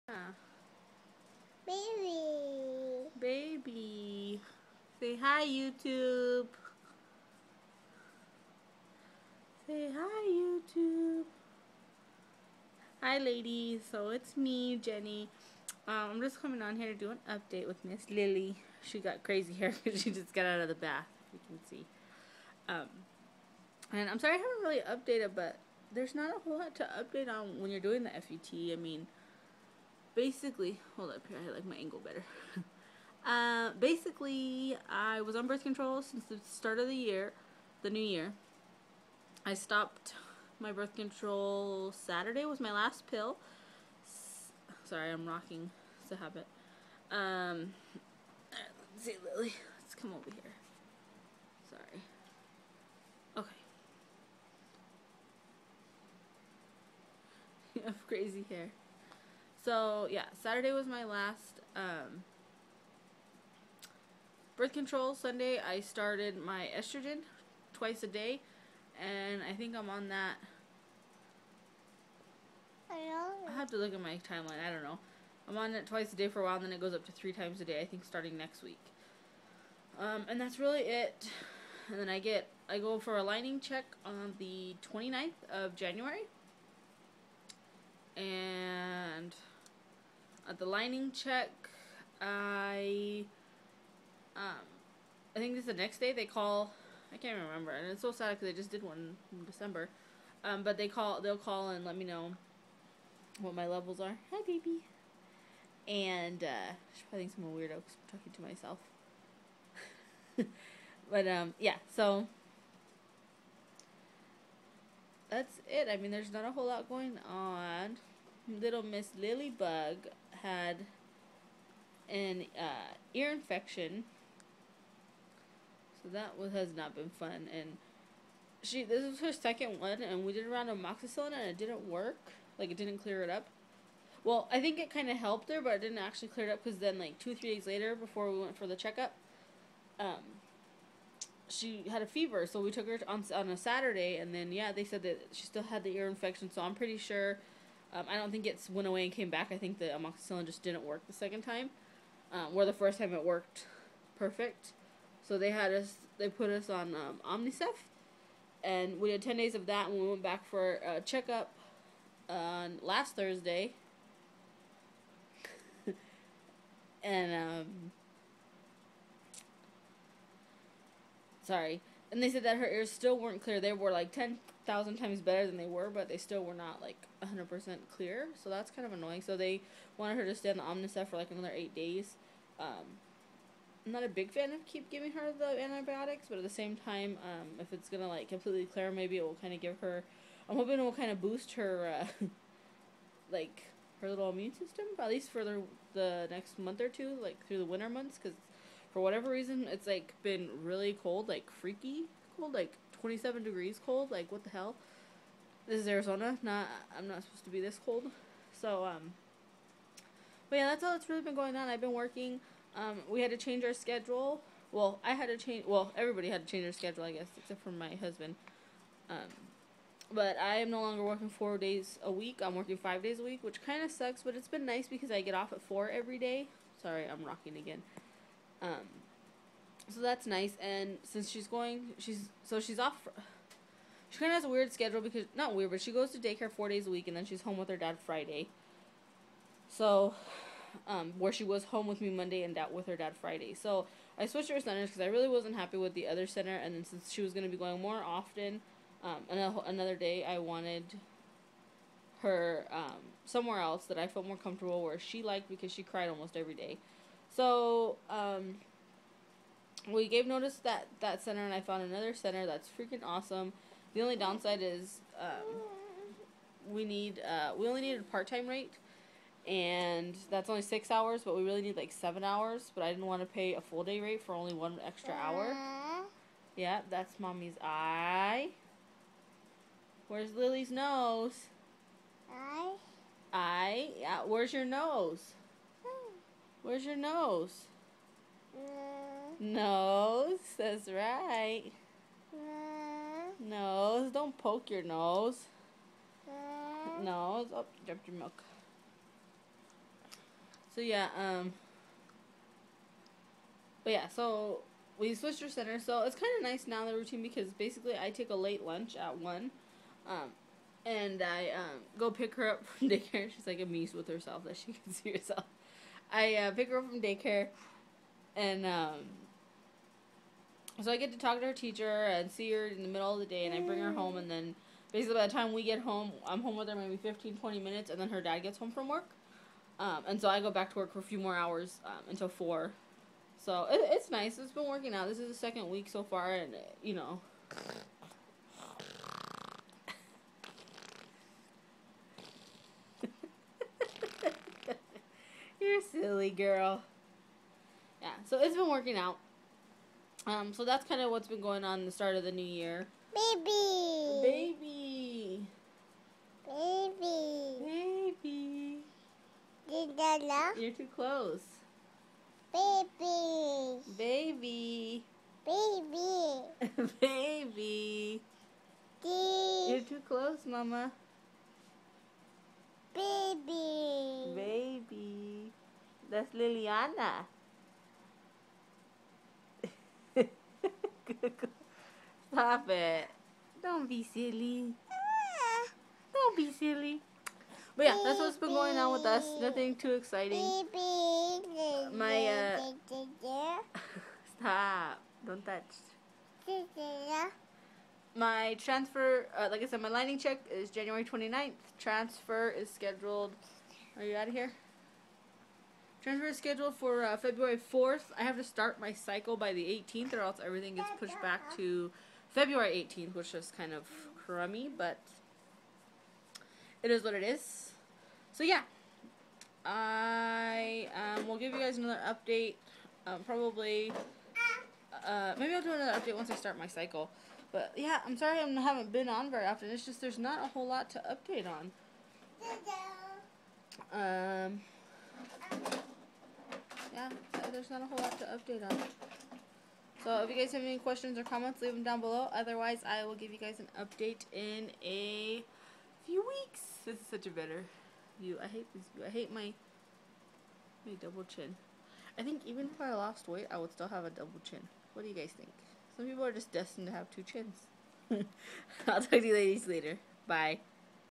Fet update
Just a cycle update and a little bit about what's going on these days